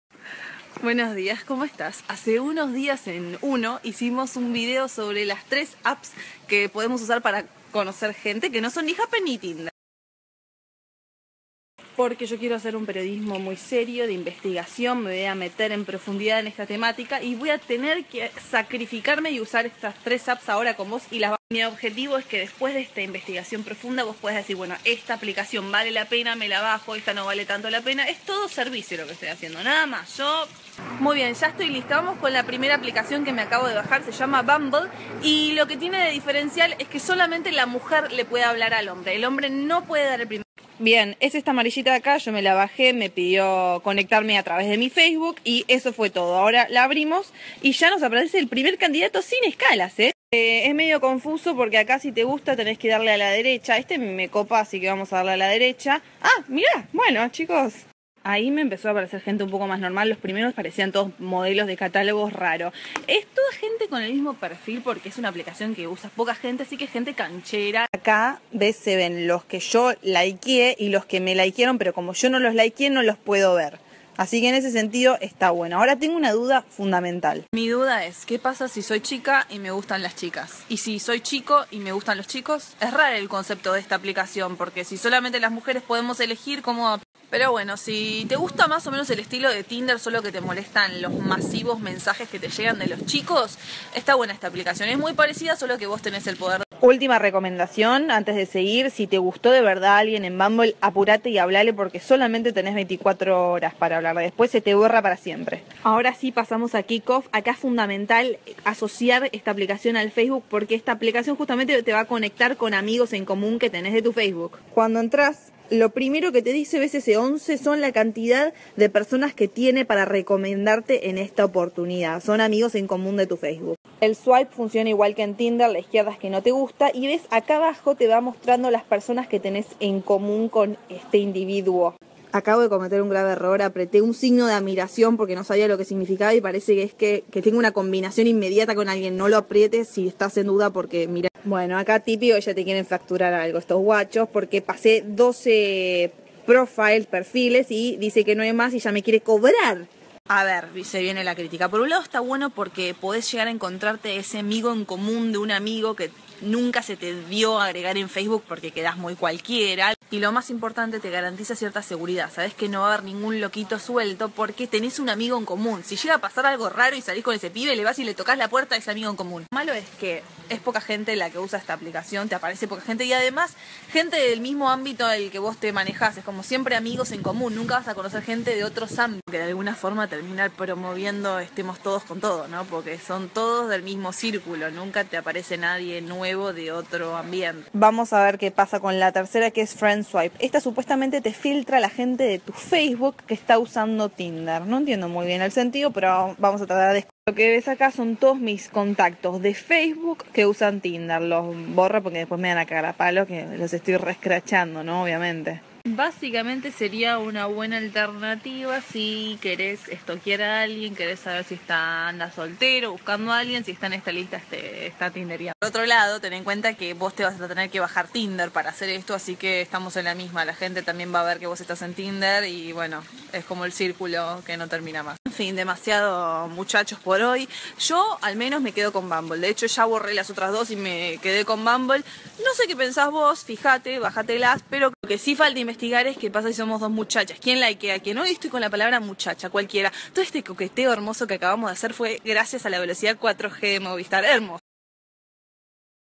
3 apps para levantar
Mai te muestra 3 apps no tan conocidas que podés usar para conocer gente